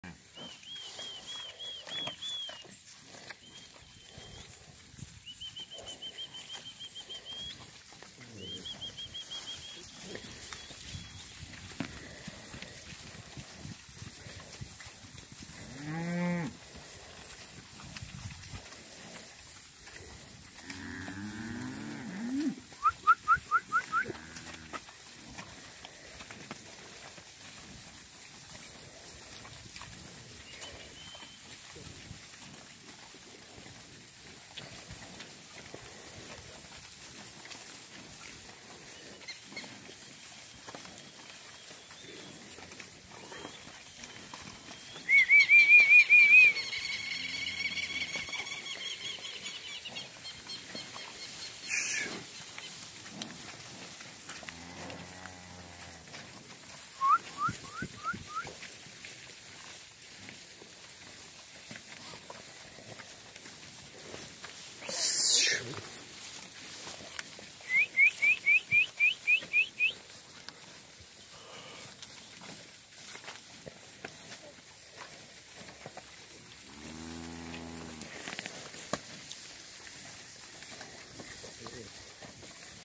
Vacas horras venta
Lote de hermosas vacas horras aptas trópico bajo, leche 100% con muy buen fenotipo, de primer y segundo parto, todas desde 5 y 8 meses de preñez.Se encuentran trabajadas con toros Gyr y Gyrolando. Las vacas en producción son de 18 y 25 litros/ día, suplementadas y buenas pasturas, excelente procedencia.